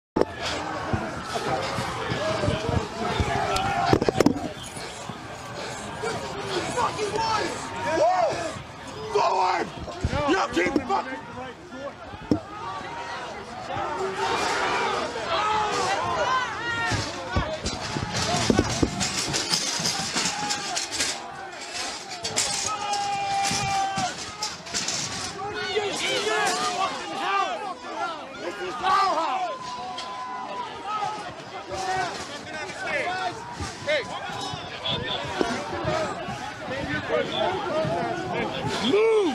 Huge crowd marches up to handful of officers at Capitol steps
January 6, 2021 - 1:49 PM